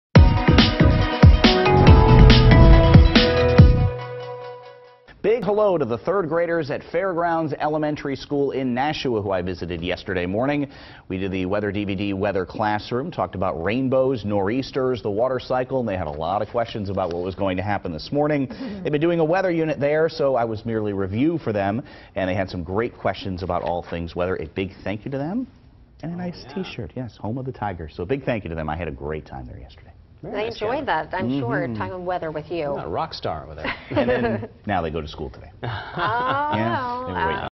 School visit: Fairgrounds Elementary School in Nashua
Meteorologist Kevin Skarupa recently visited Fairgrounds Elementary School in Nashua.